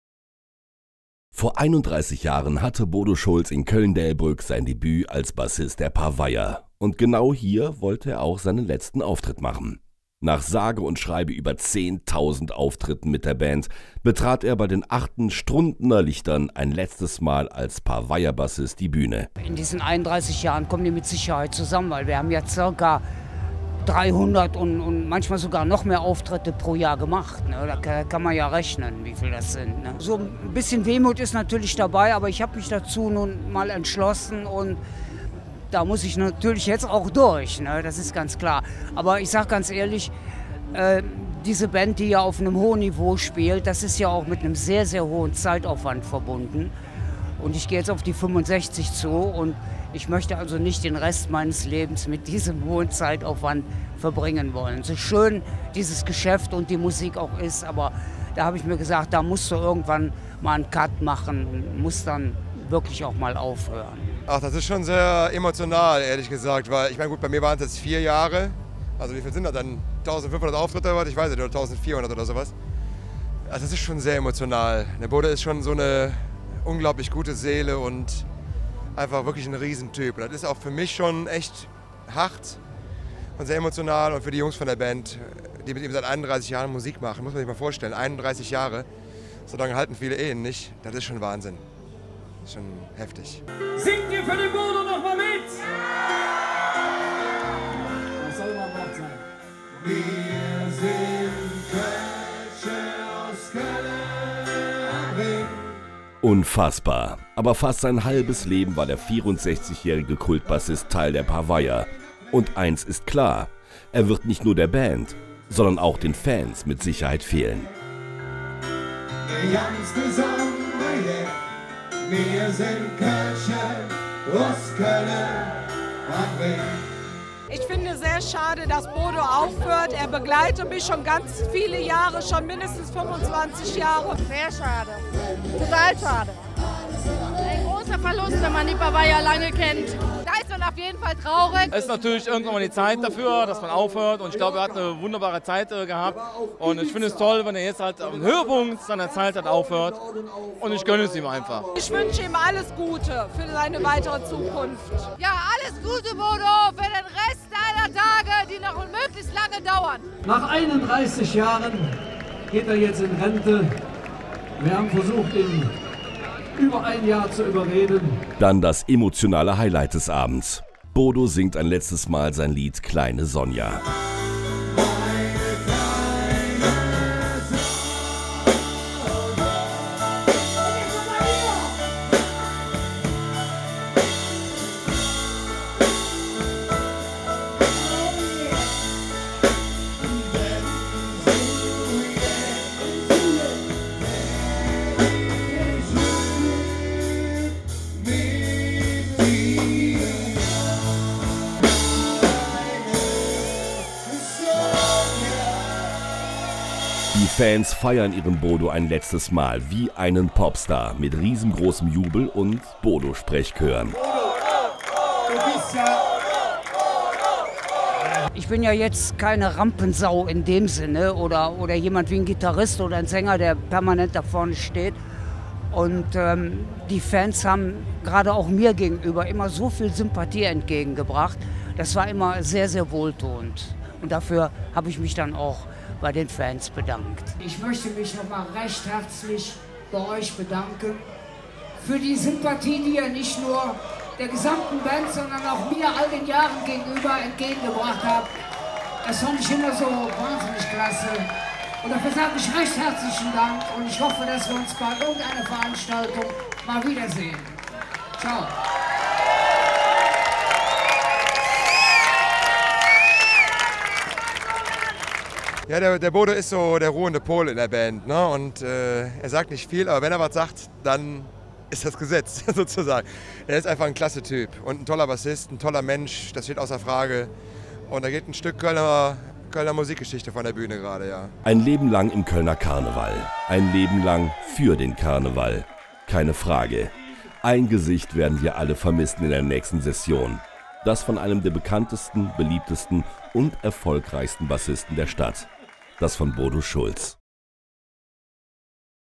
Bodo Schulz - letzter Auftritt als Paveier-Bassist
Der letzte Auftritt eines Urgesteins: Wir waren dabei als Bodo Schulz, Kult-Bassist der Paveier, sich von den Fans verabschiedete.